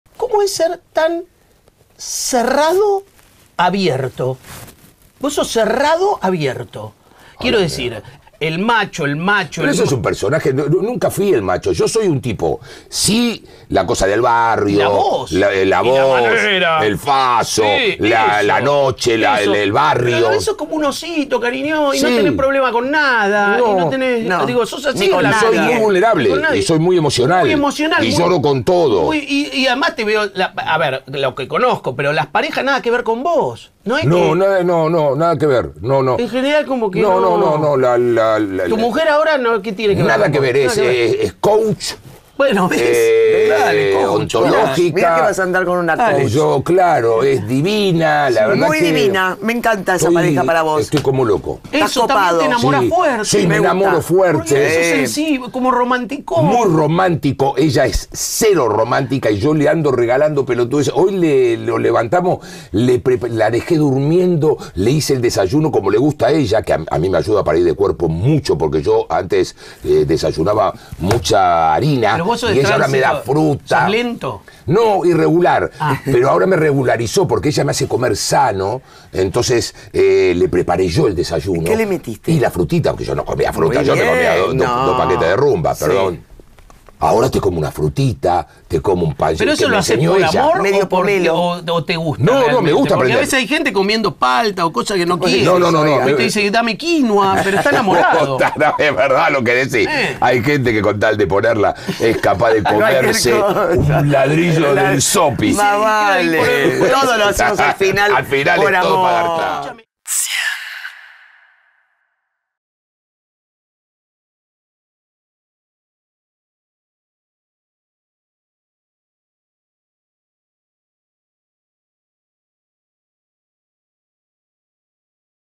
🔴COCO SILY: "Soy muy emocional y vulnerable" 🔴
🔴  Coco Sily pasó por La 100 y habló de todo con Guido Kaczka y Claudia Fontán.

👉 La primera parte de la nota está ACA 👉    • ⭕ COCO SILY con Guido Kaczka y Claudi...

👉 La segunda parte de la nota está ACA 👉    • 🔴 COCO SILY con Guido Kaczka y Claudi...